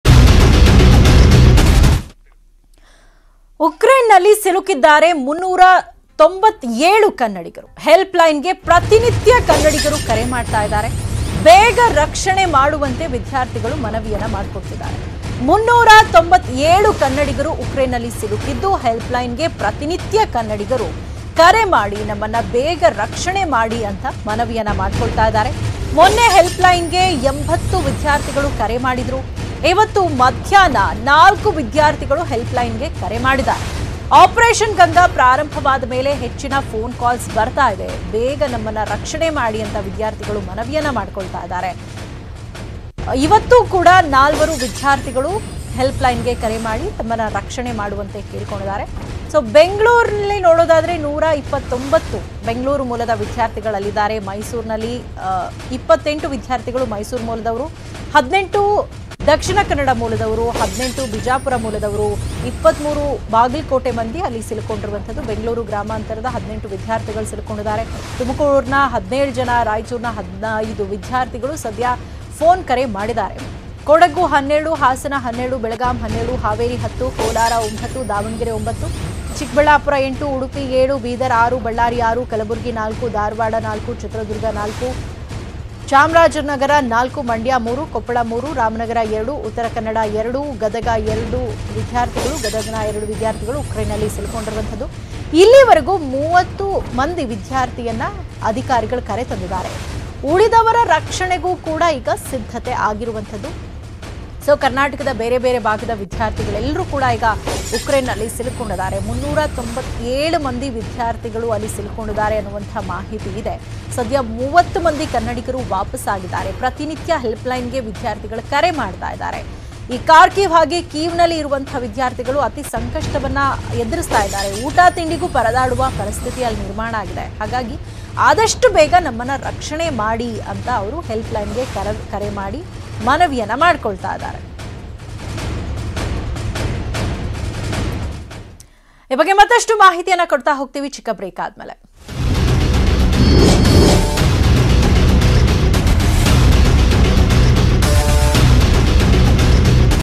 Russia-Ukraine War| ಉಕ್ರೇನ್ ನಲ್ಲಿ ಇನ್ನೂ ಸಿಲುಕಿದ್ದಾರೆ 397 ವಿದ್ಯಾರ್ಥಿಗಳು; ರಕ್ಷಣೆ ಮಾಡುವಂತೆ ಮಕ್ಕಳ ಮನವಿ
Russia-Ukraine War |   ಉಕ್ರೇನ್ ನಲ್ಲಿ ಇನ್ನೂ ಸಿಲುಕಿದ್ದಾರೆ 397 ವಿದ್ಯಾರ್ಥಿಗಳು. ಹೆಲ್ಪ್ ಲೈನ್ ಗೆ ಕರೆ ಮಾಡಿ ಬೇಗ ರಕ್ಷಣೆ ಮಾಡುವಂತೆ ಮಕ್ಕಳ ಮನವಿ ಮಾಡಿಕೊಳ್ಳುತ್ತಿದ್ದಾರೆ.